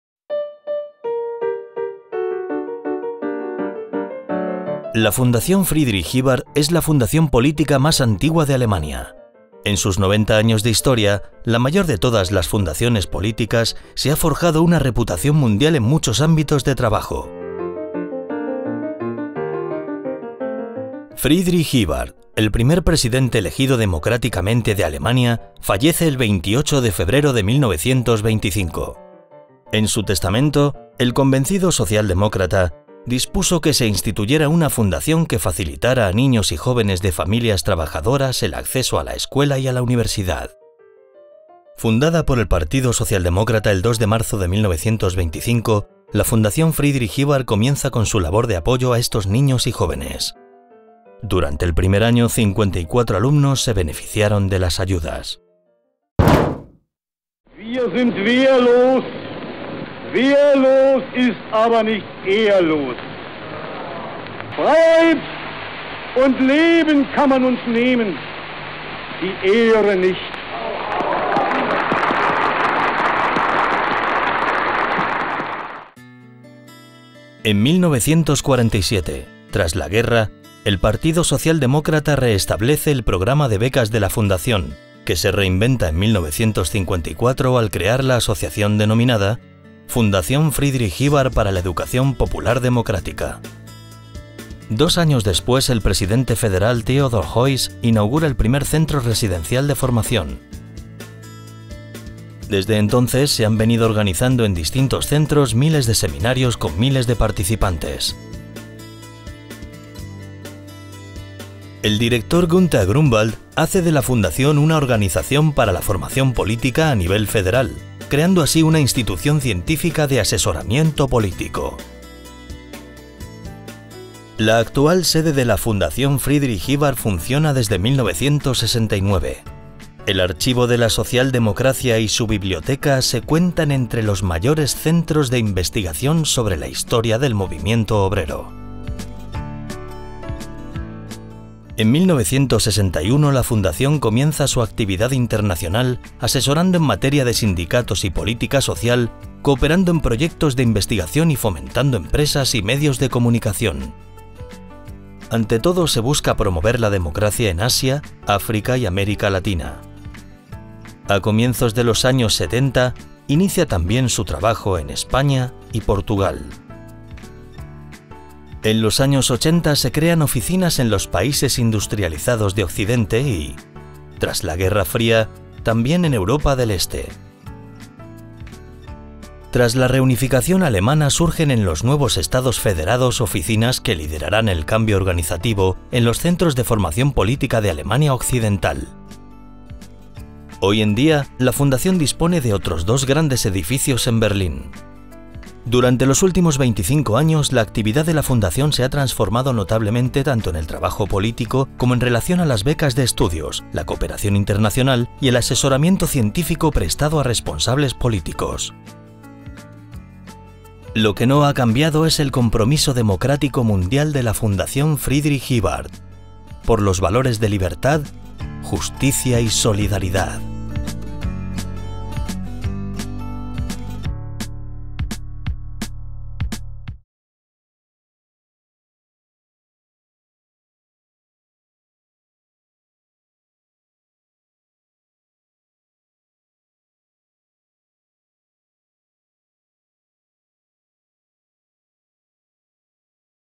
90 años de la Friedrich-Ebert-Stiftung
El video muestra en 4:04 minutos la historia de la Friedrich-Ebert-Stiftung, desde su creación, el 2 de marzo de 1925, hasta el presente.